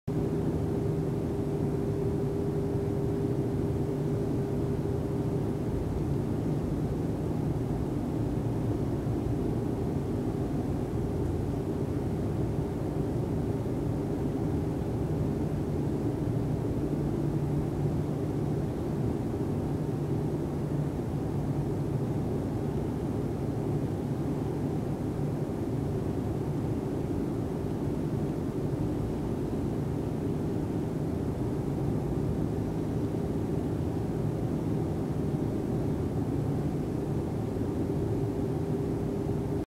2011年7月15日 アクアマリンふくしまの展望室
景色はいいけど暑い